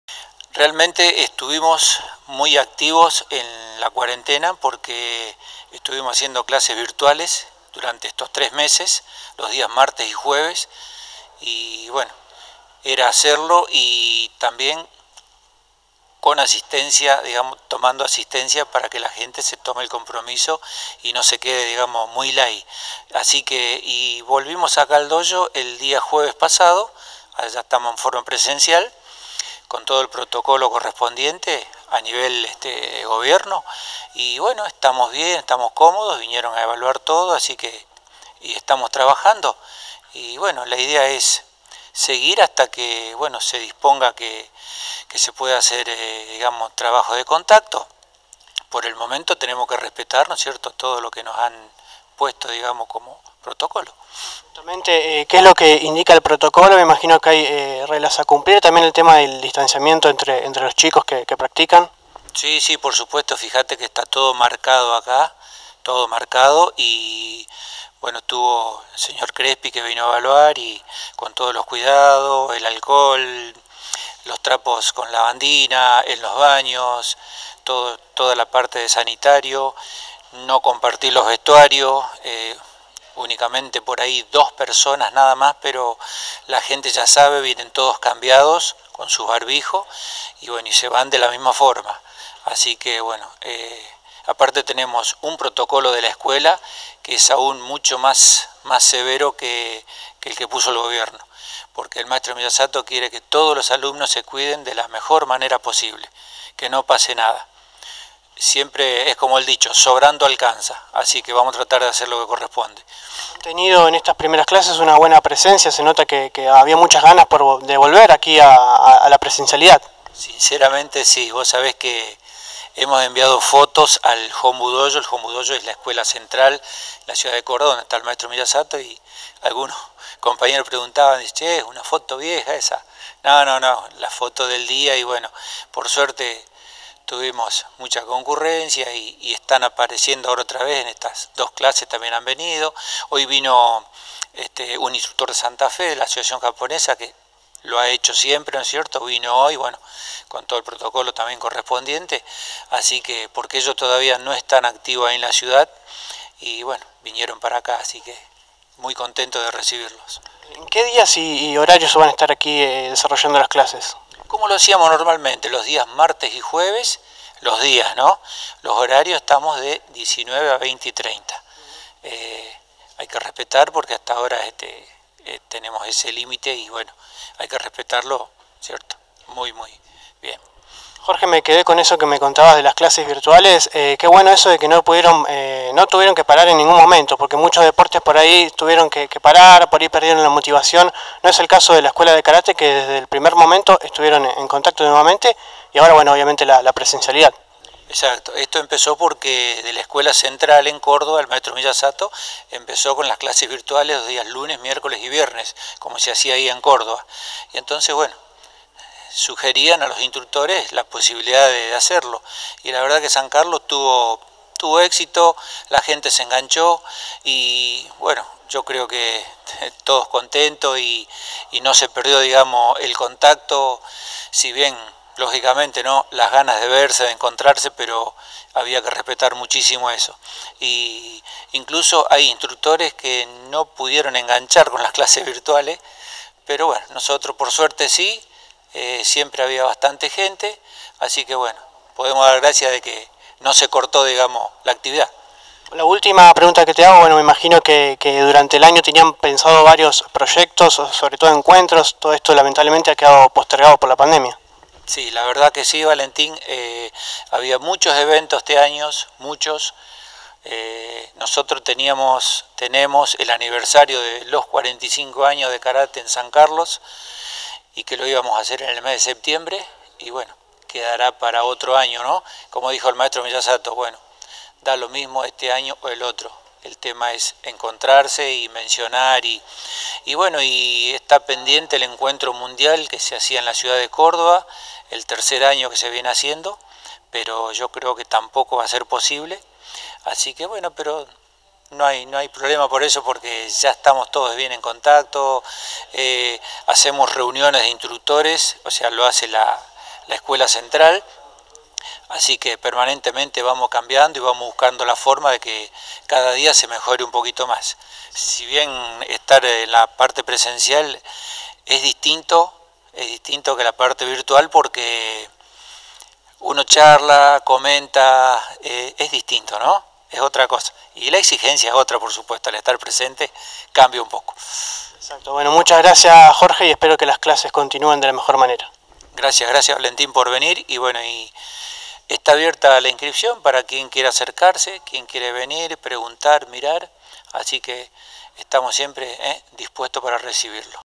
Jorge Barbaglia - Esc. Miyazato de Karate Do
Gran concurrencia en la vuelta de las clases en el Dojo